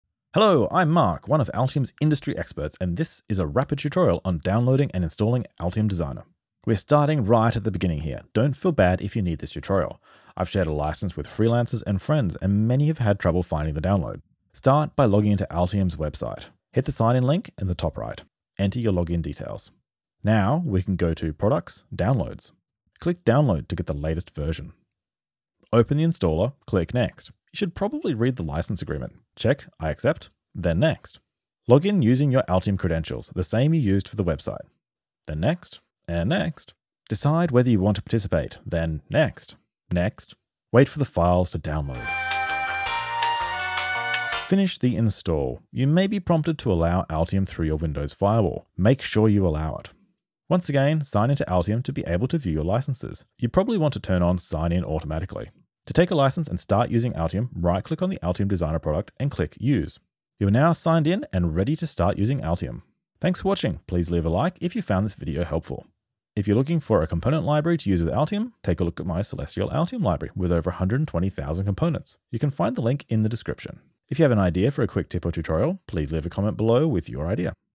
Altium Rapid Tutorial - Downloading Installing Altium Designer
Learn how to download, install, and license Altium Designer in this Rapid tutorial. There's no messing around, just straight to the point as fast as possible!